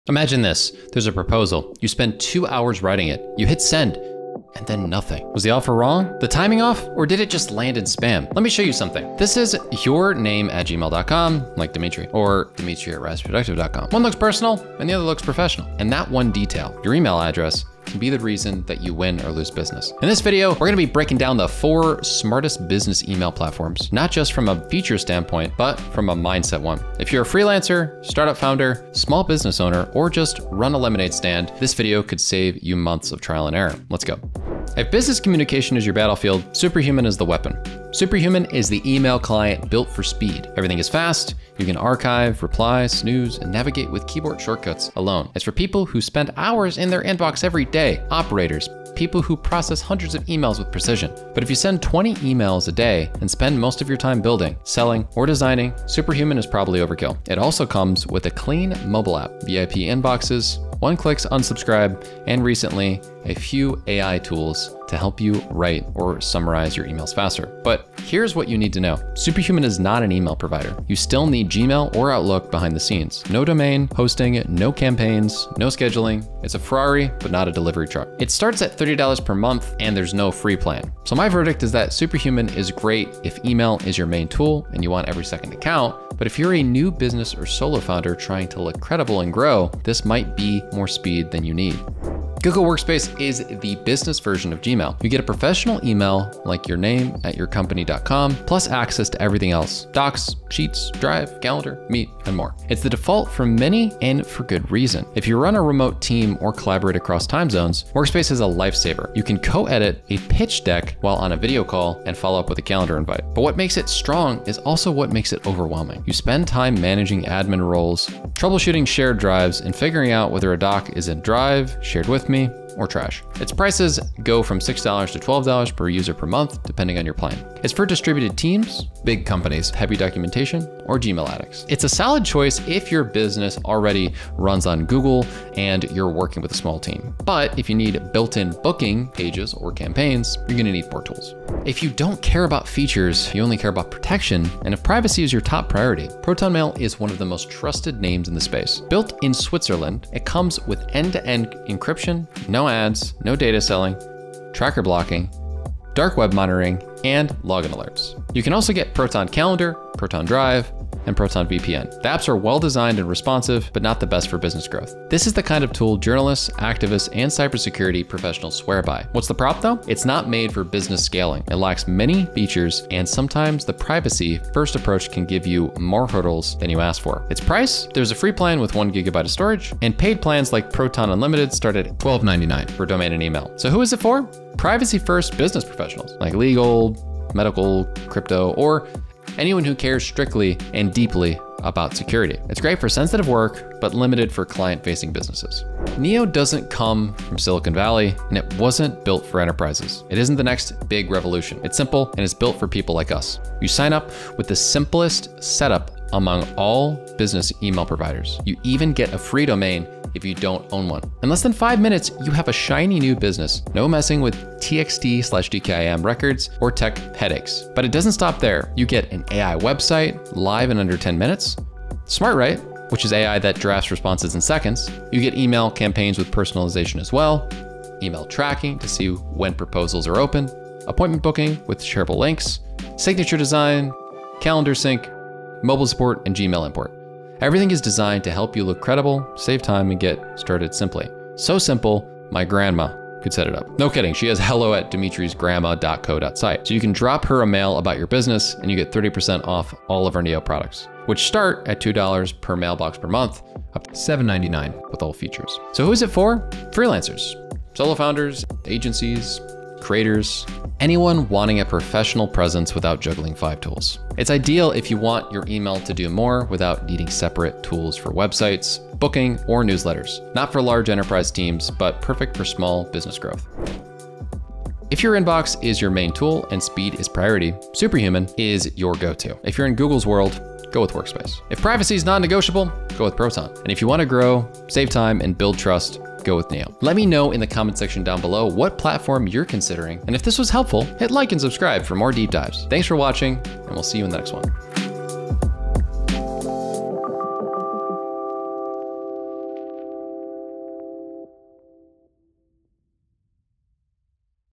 Best Email Hosting of 2026? | Google Workspace vs Superhuman Mail vs Proton Mail vs Neo
Looking for the best email hosting of 2026 for your small business? We will review Google Workspace, Superhuman Mail, Proton Mail, Zoho Mail and Neo.

We will explore the best email hosting for your small business by reviewing features, pricing, and usability so you can choose the right platform to look professional and stay productive. Having a custom domain email is essential for building trust with clients and standing out from the competition.

Stop relying on free Gmail or Yahoo accounts. It is time to upgrade to a professional email service that matches your business.

⏰ TIMESTAMPS
00:00 Intro
00:34 Superhuman Email
01:32 Google Workspace
02:21 Proton Mail
03:15 Neo
04:34 Summary
04:47 Outro